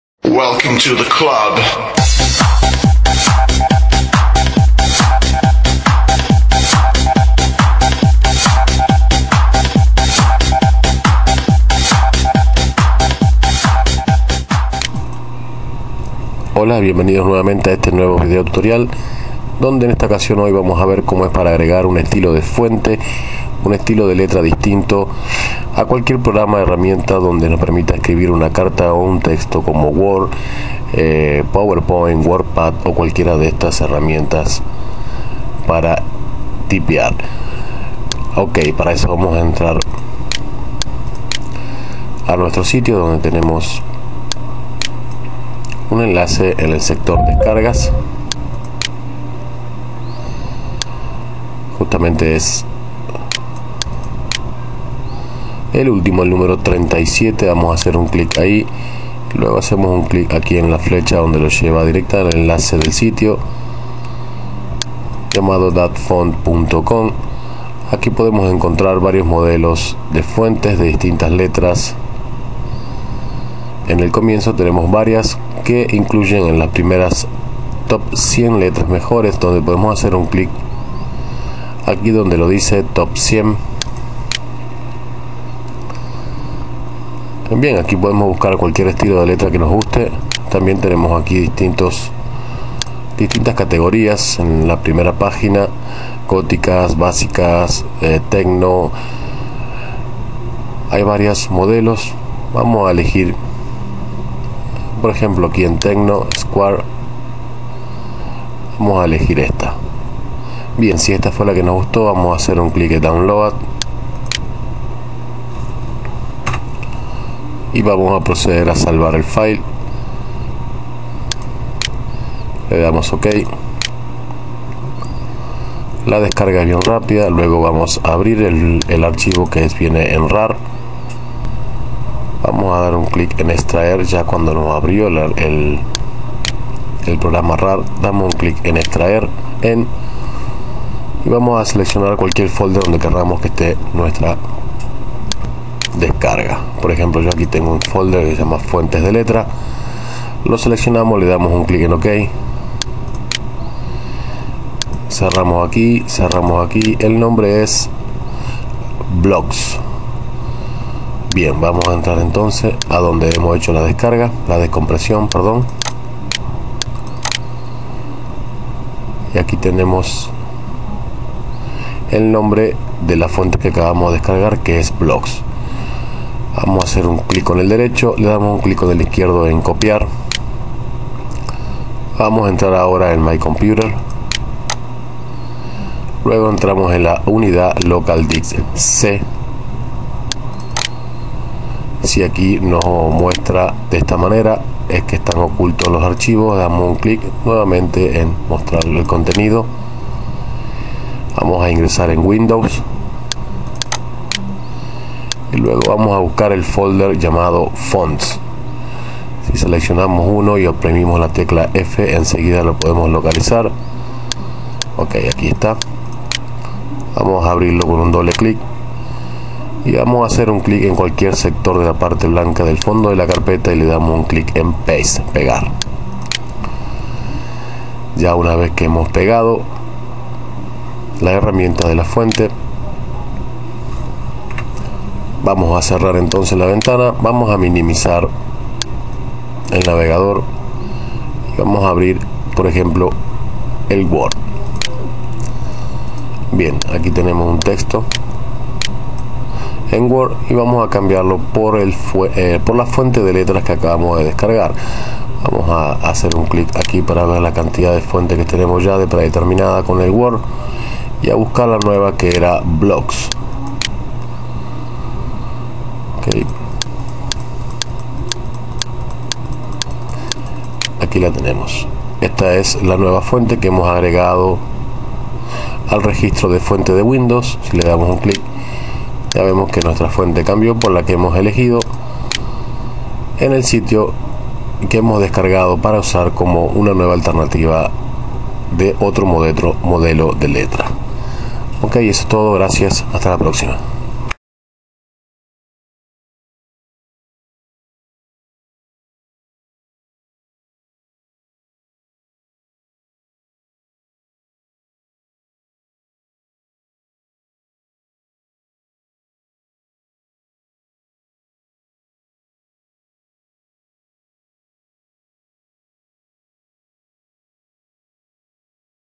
Descargar distintas fuentes de letras online
En este sitio web puede encontrar distintos modelos de fuentes en letras para su texto o edicion .